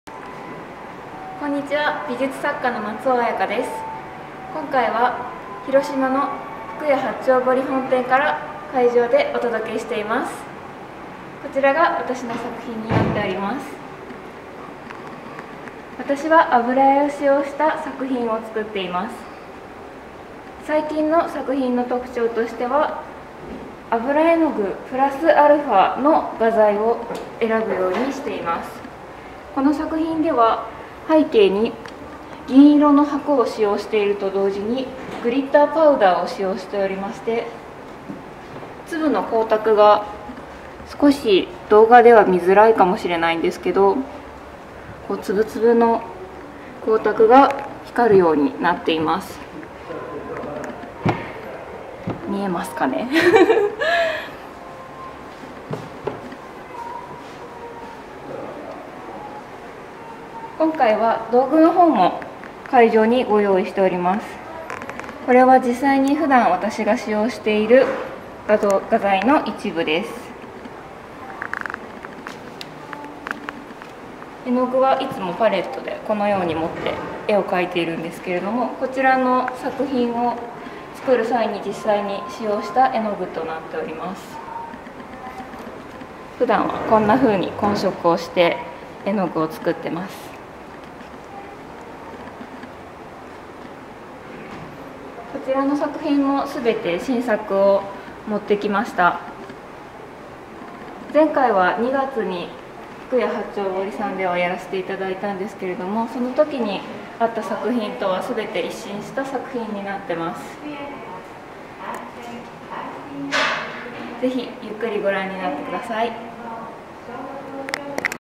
金銀箔展（中国地方・広島）：松尾彩加による作品解説（全体）
福屋八丁堀本店で始まりました金銀箔展ファイナルの動画です。
油彩画家の松尾彩加の最初の解説は全体的な技法やテーマなどについてです。
是非、ご自宅でお楽しみください！